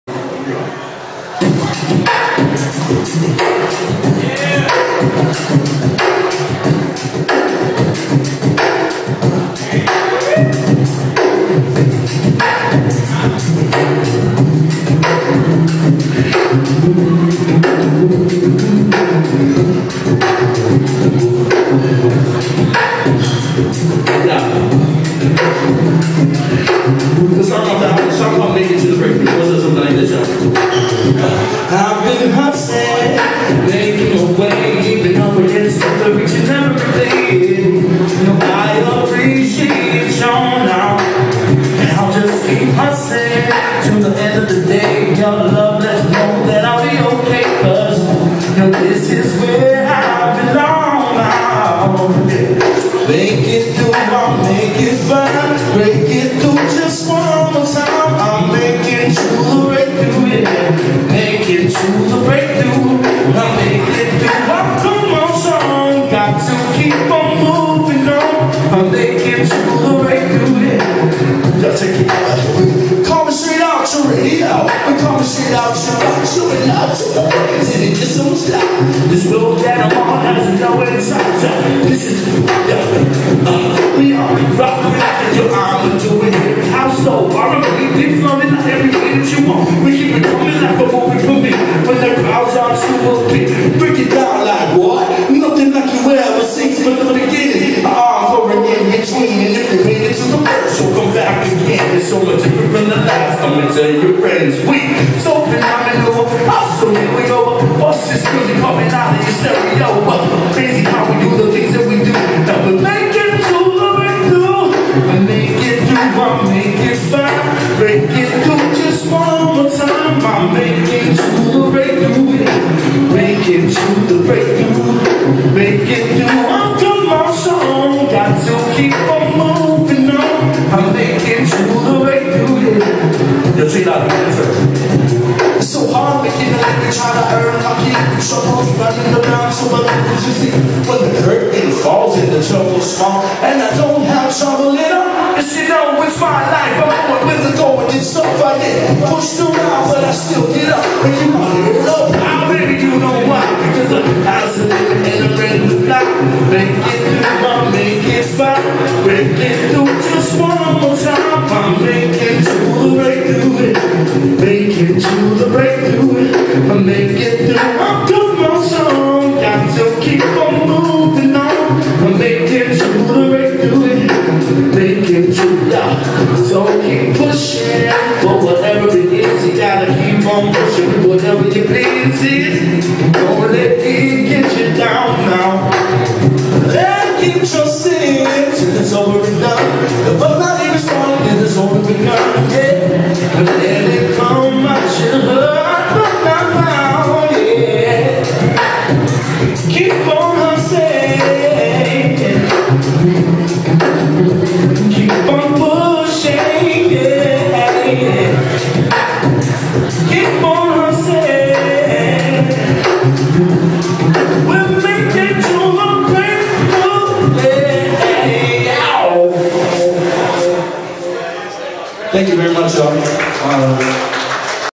8/22/09 Cityside Cd Release Party @ Oceans Kihei pt.2
Kanoa of Gomega opening act